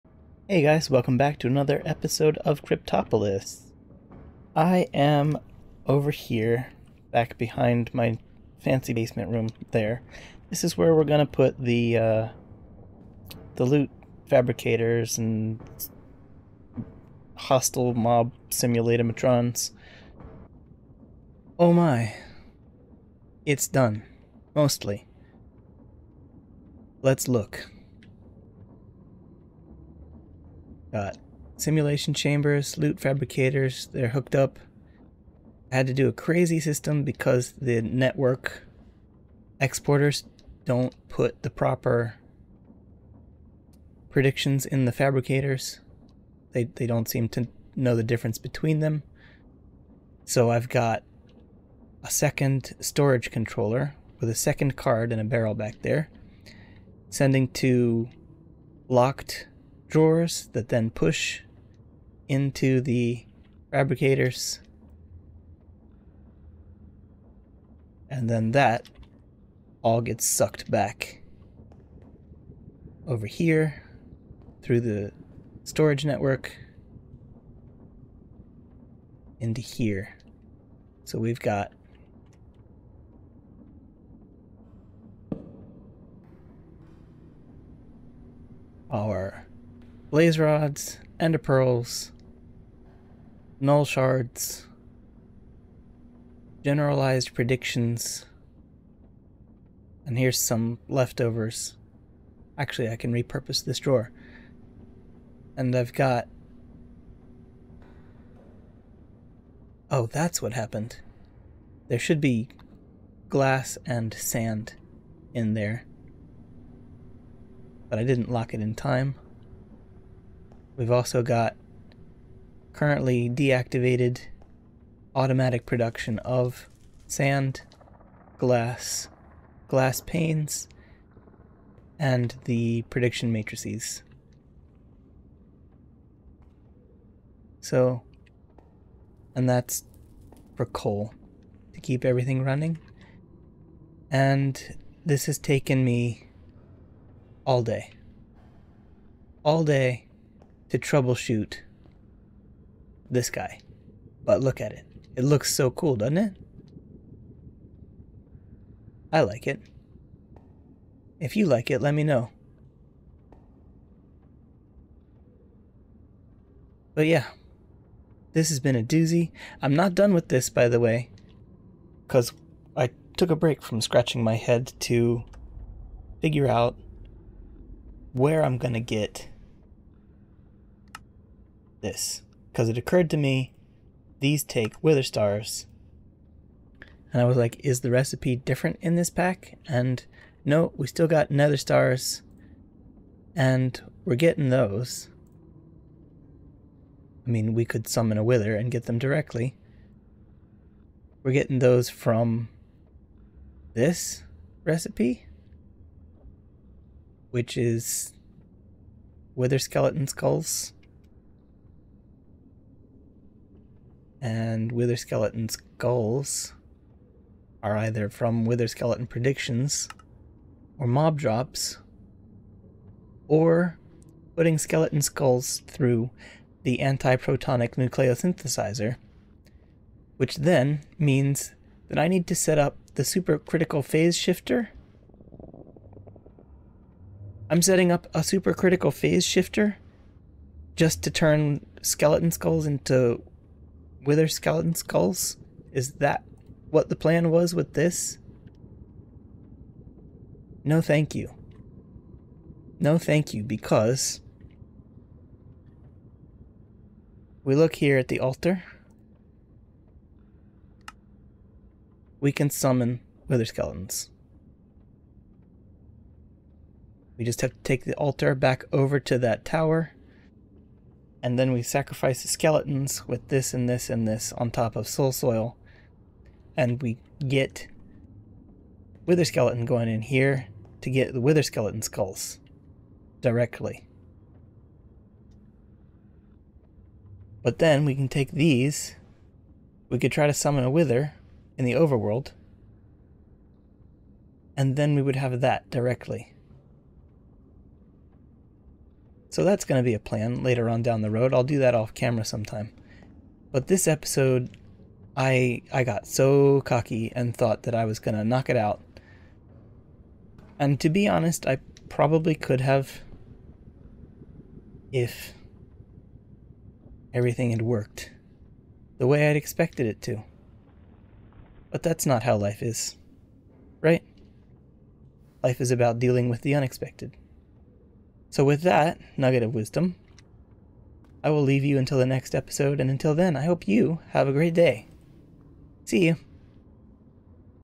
Why so Hostile? - Cryptopolis Ep 14
I got the Hostile Neural Network all set up... after about five hours of troubleshooting with getting the loot fabricators to play nice with my item network.

Sorry for the short video - as I said, this took absurdly long, and I'm very busy IRL this time of year. It was mostly footage of me trying to get a filter system to work, so you didn't miss out on much.